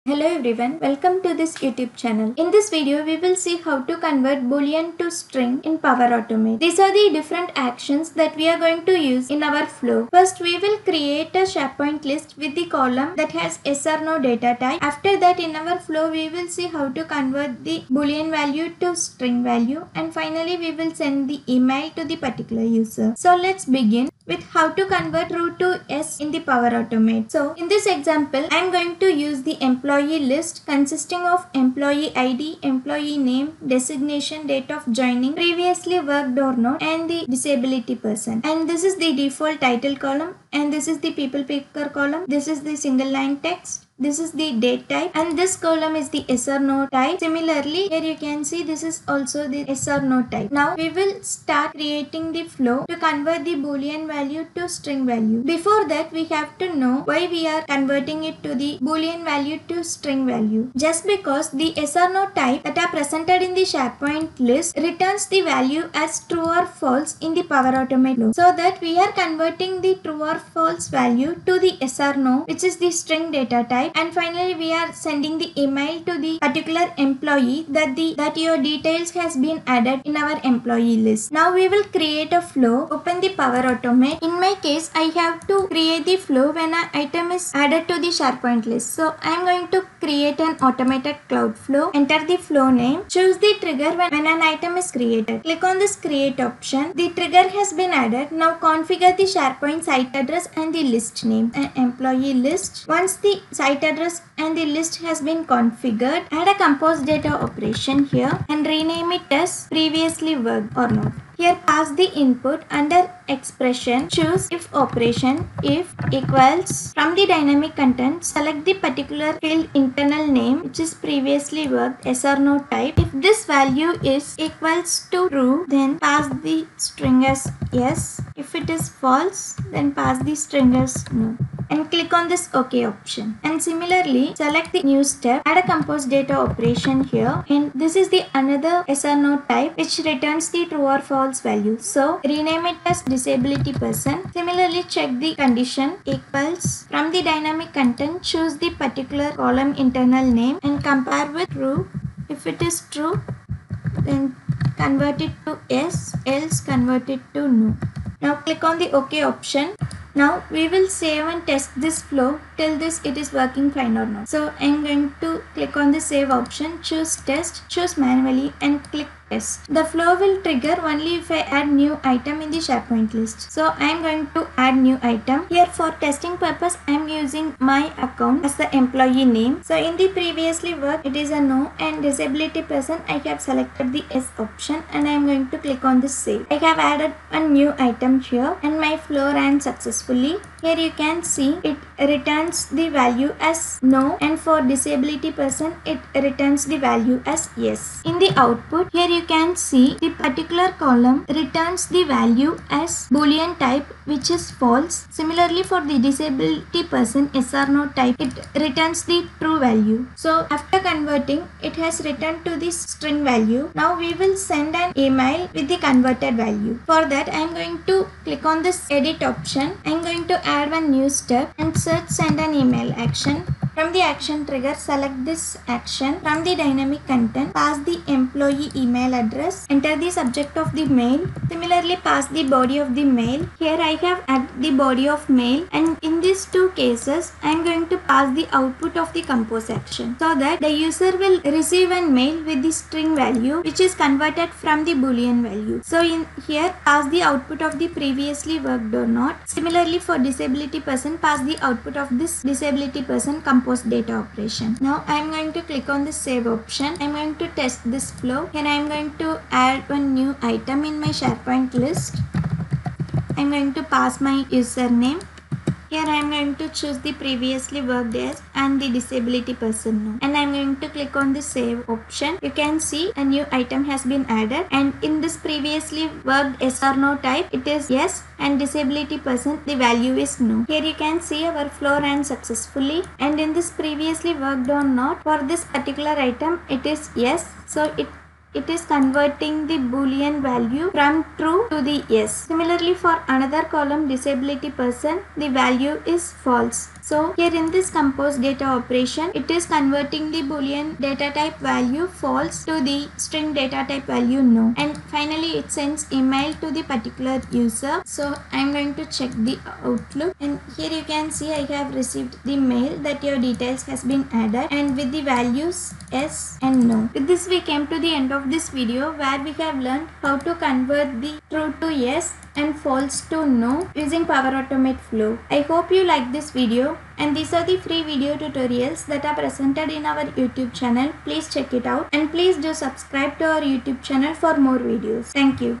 How to Convert Boolean to String in Power Automate | Convert True/False to Yes/No in Power Automate
In this Microsoft Power Automate video, I have explained how to convert True to Yes and False to No in Power Automate Flow.
Before that, we have to know why we are converting True to Yes and False to No in Power Automate.
PowerAutomate Flow doesn't recognize a SharePoint Yes/No Column value and returns true or false, but we may in need to display the exact output value as Yes /No.
In those cases, we want to convert the default value True to Yes and False to No. So here I have clearly shown you how to convert True to Yes in Power Automate flow. It helps you to convert the incoming Yes or No value to its corresponding true or false value.
Finally, once the flow is triggered it sends an email to the particular user with the converted string value from the boolean value.
Check the complete tutorial on How to Convert True to Yes in Power Automate